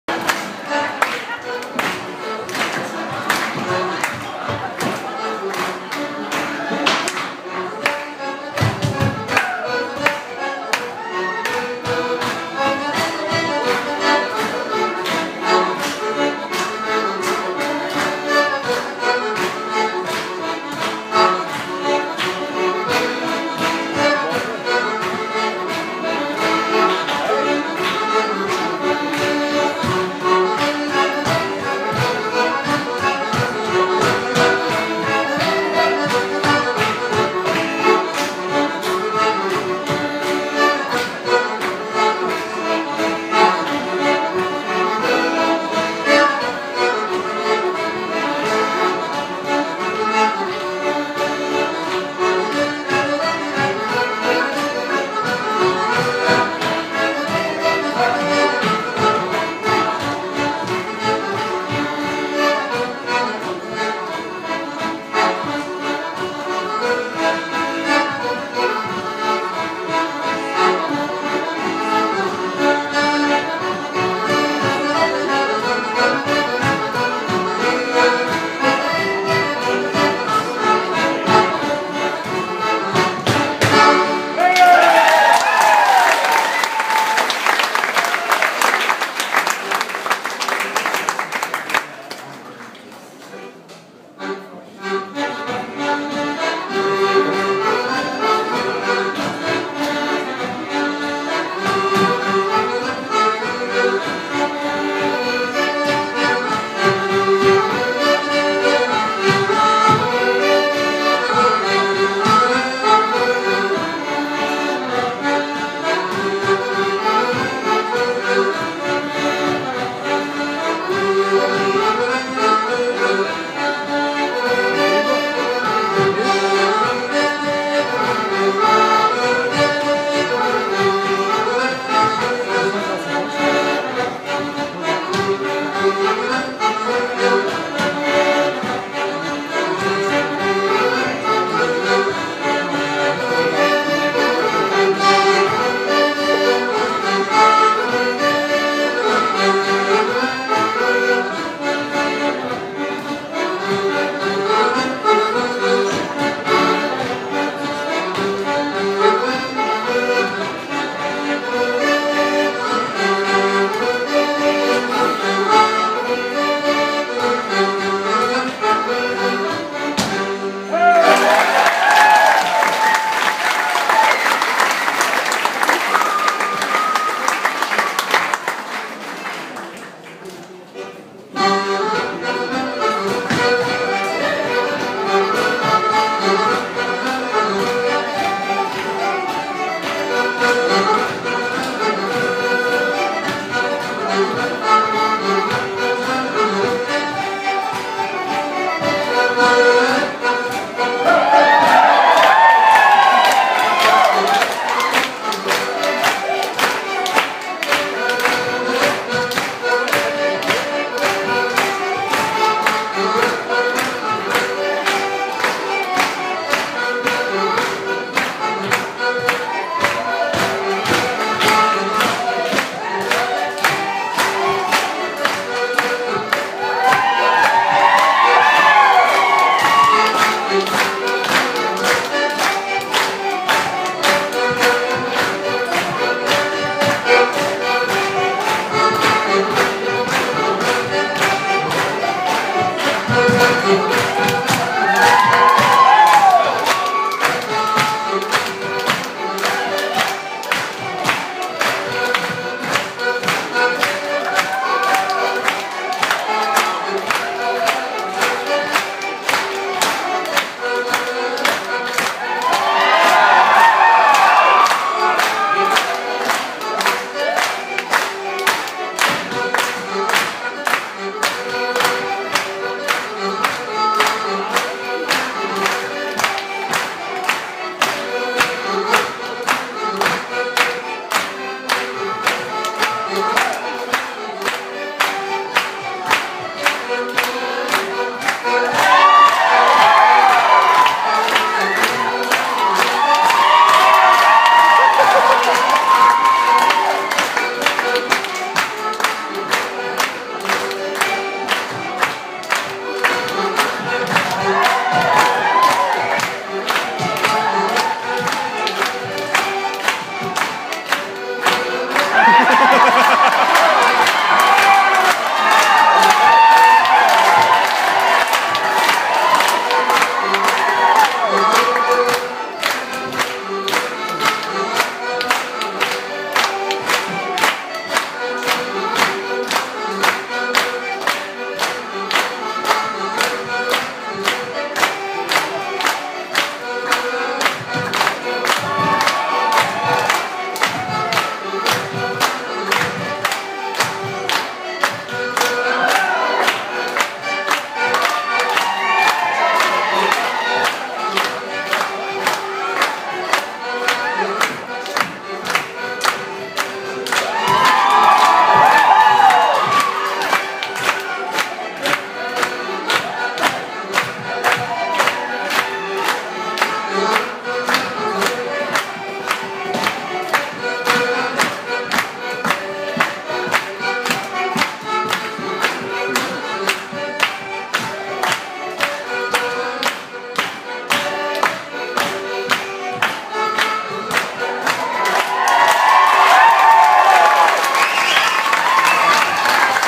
Charity Jigs & Reels with Louise Kelly & Tony Doherty
Jigs & Reels with Louise Kelly & Tony Doherty (Mickey John) @ the Ballyliffin Hotel in aid of Breast Cancer & The Cancer Bus